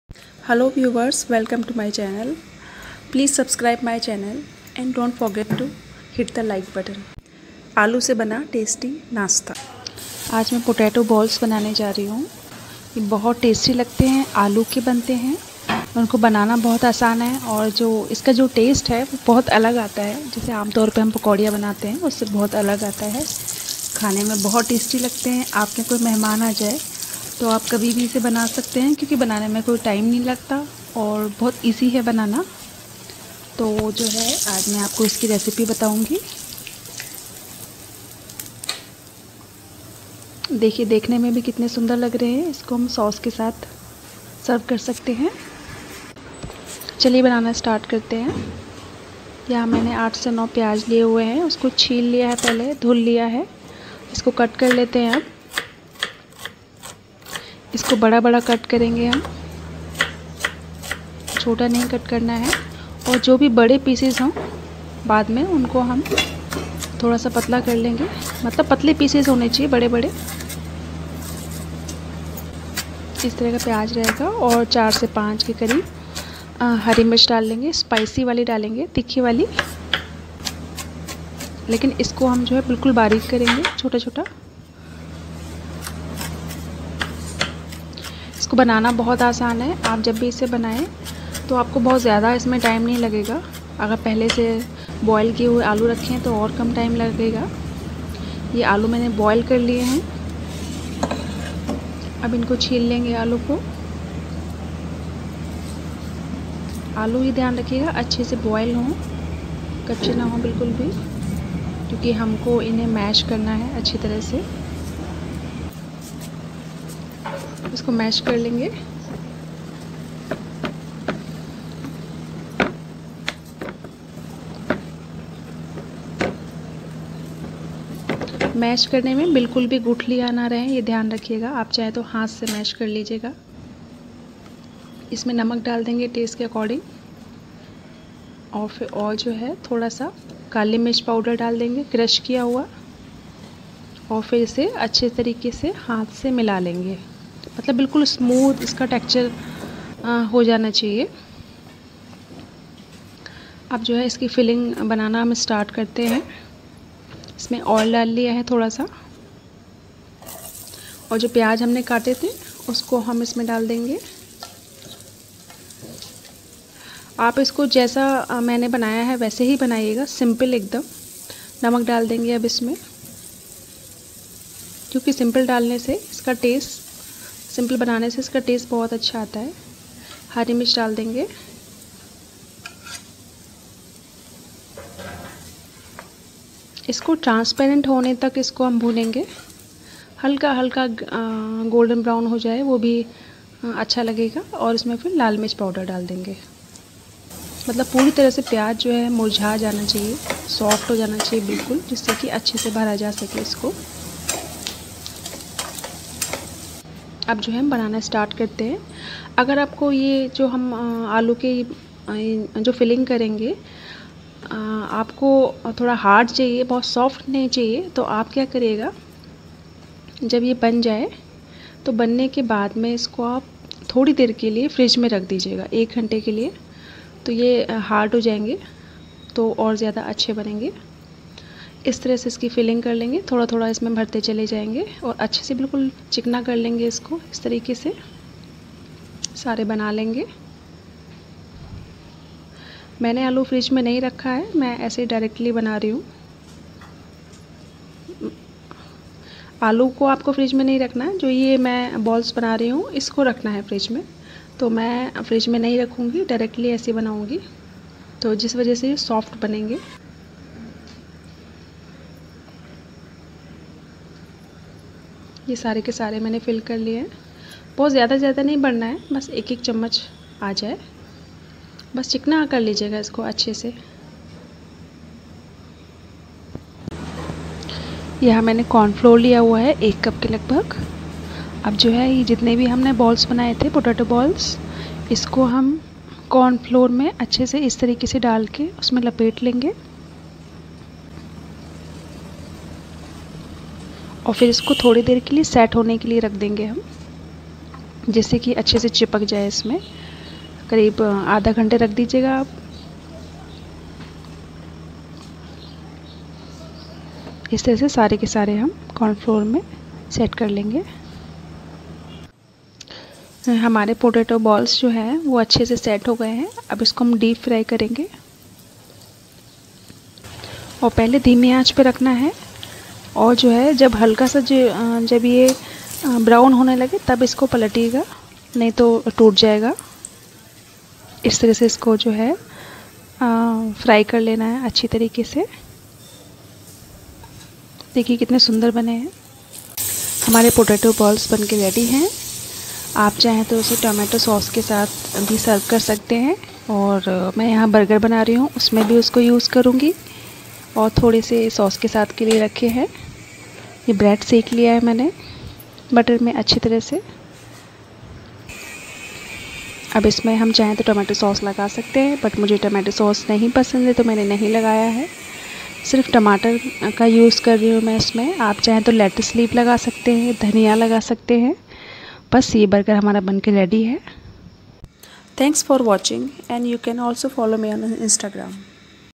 आलू के टेस्टी रोल Street Food Recipe | Hot and Spicy Stuffed Potato Roll | Onion potato Cutlets
Potato Cutlets

 Ingredients

Boiled Potatoes
Onion, Green Chilli
Cumin seeds
oil
Corn flour
Salt